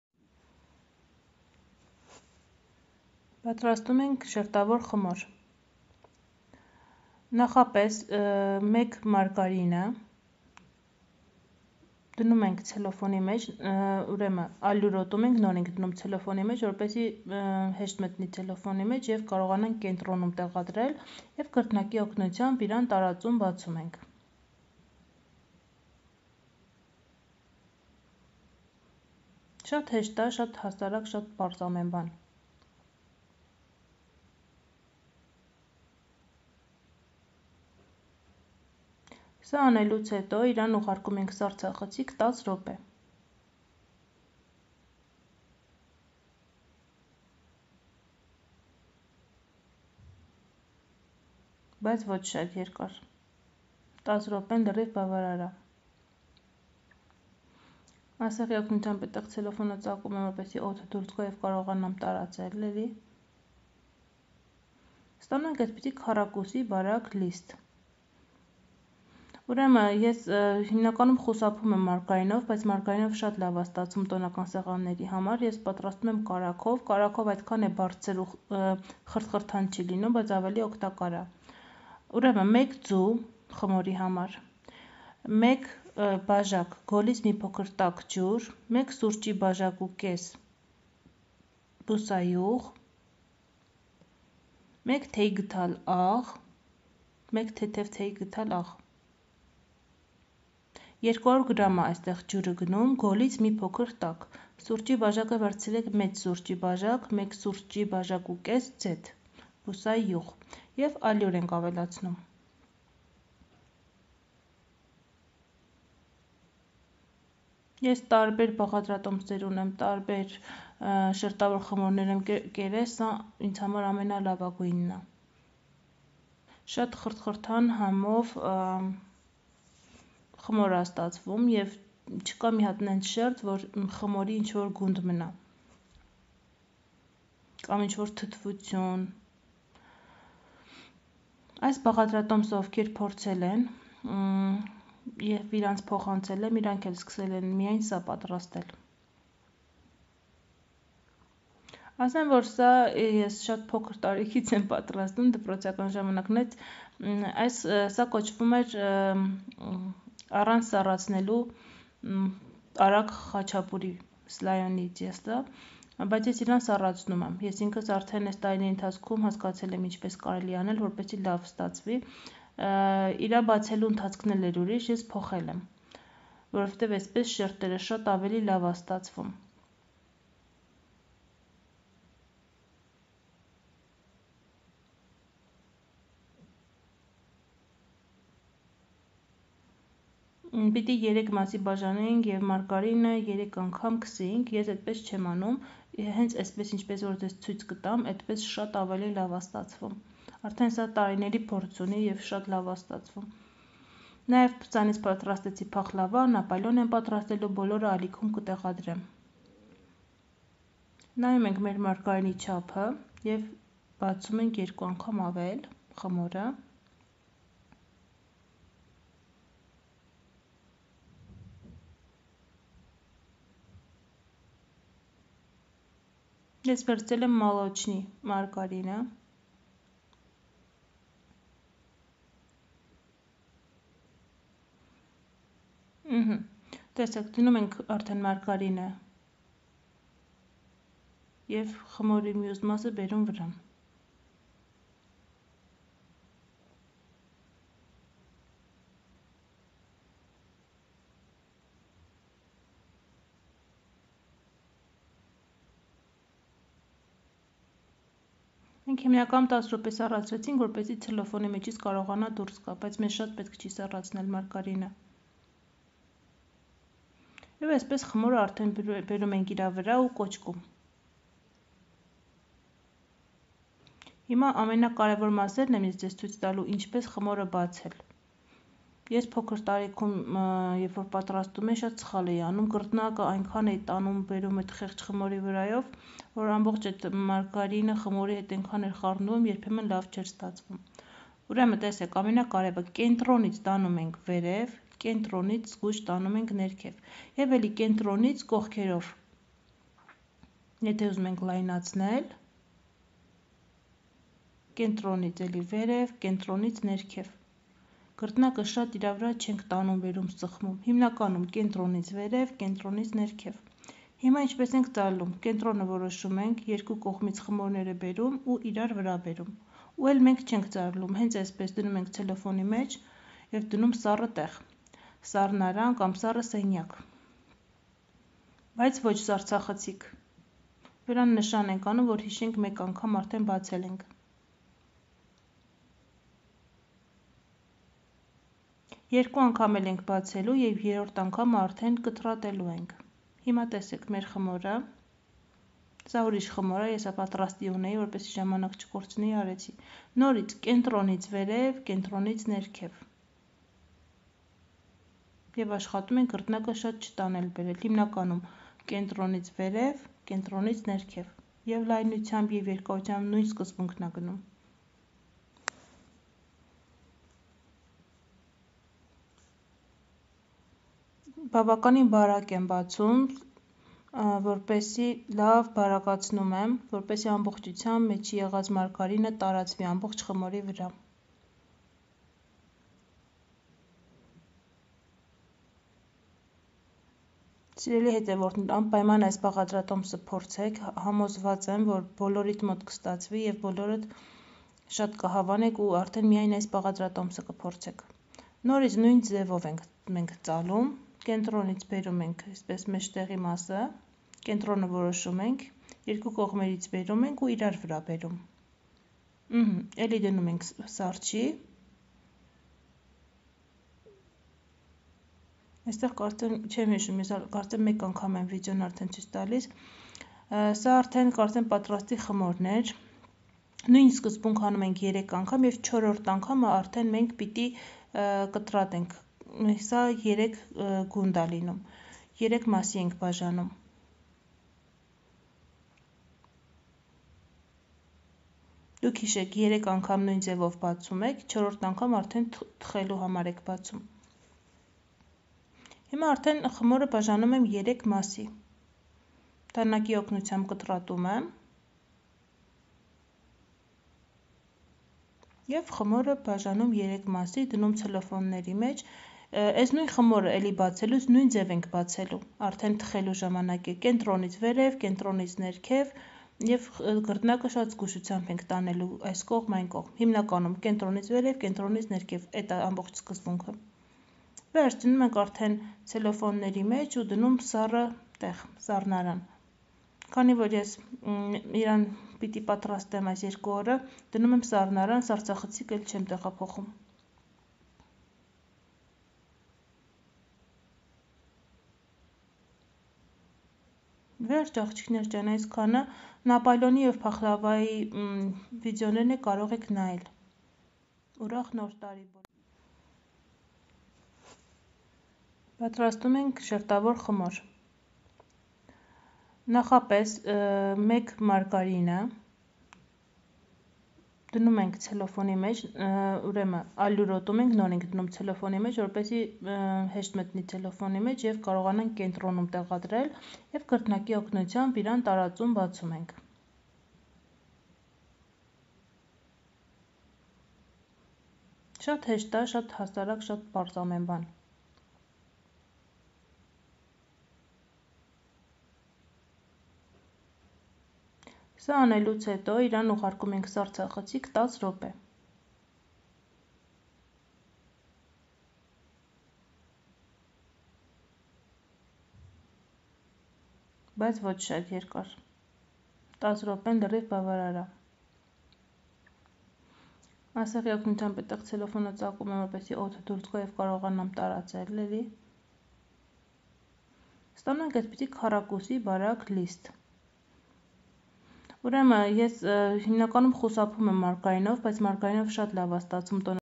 շերտավոր խմոր/слоёное тесто
1ձու+ 200մլ գոլից մի փոքր տաք ջուր+1.5 սուրճի բաժակ բուսայուղ+ 1 թեթև թեյի գդալ աղ+ այլուր+250 գրամ մարգարին։